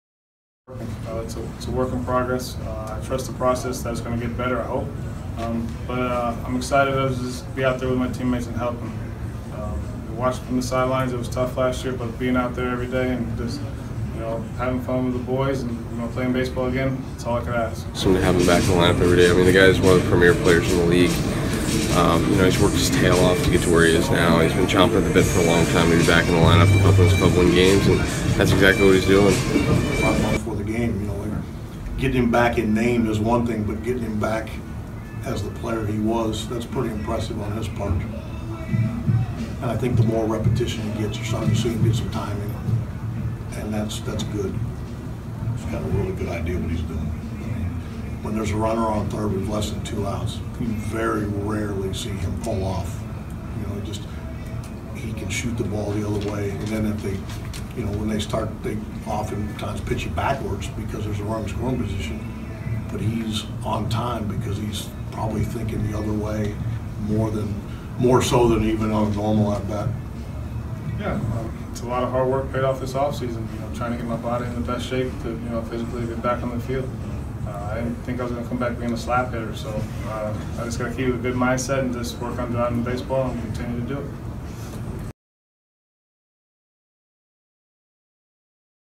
Cleveland Indians elated to have the old Michael Brantley back in the fold
Michael Brantley hasn't looked like a hitter who missed a year because of shoulder injuries.